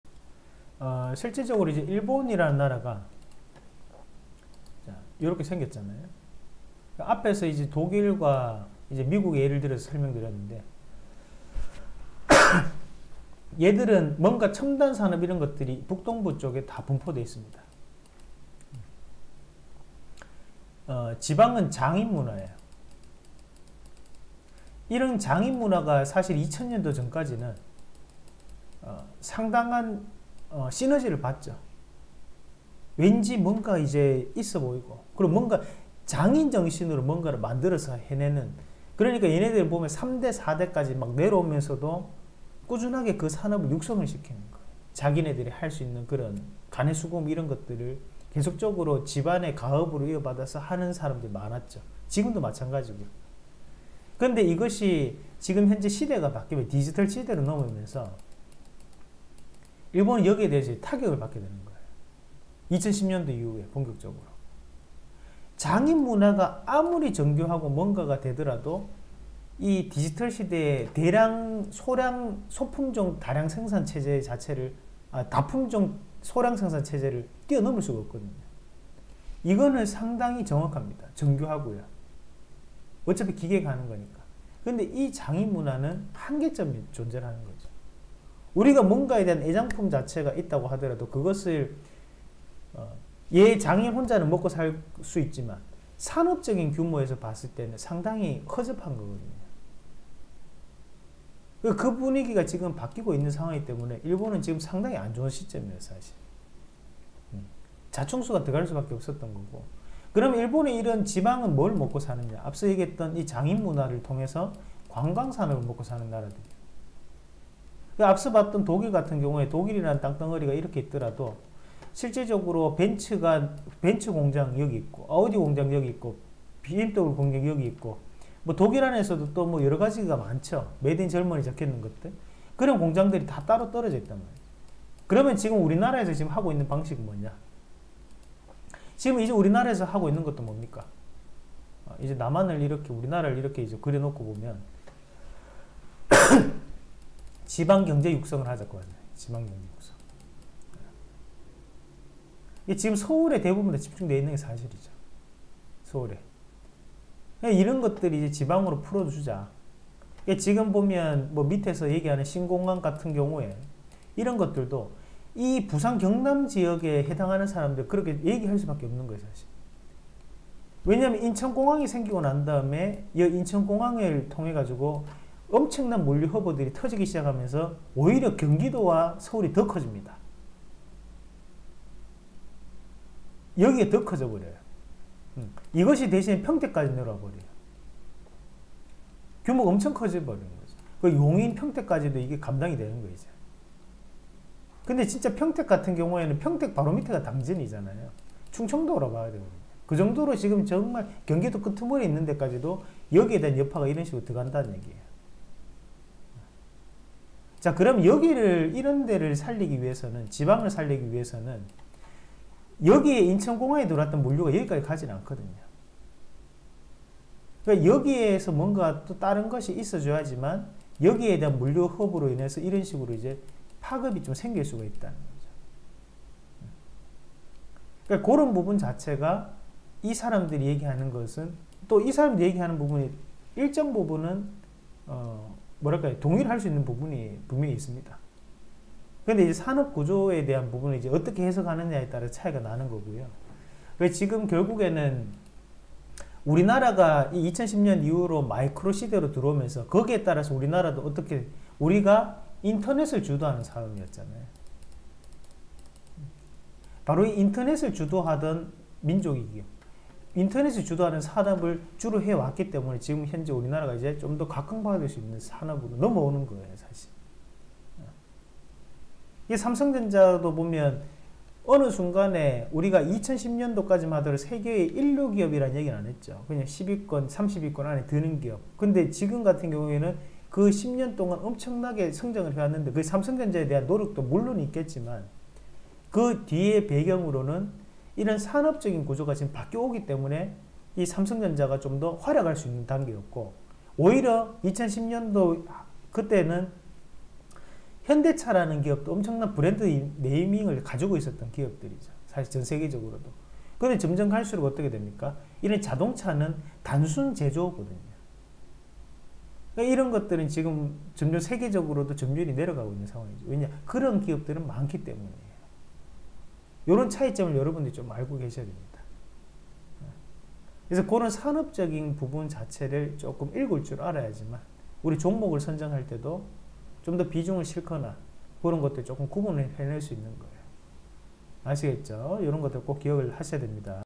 일본의 장인정신 산업이 망할 수밖에 없는 이유(feat. 독일, 미국의 산업구조, 한국의 디지털산업구조) 한국경제tv 이상로의 차트분석 & 주식공부
1. 일본은 시대를 역행하고 있다?
2. 일본의 장인정신, 이제는 한국의 디지털앞에 안된다.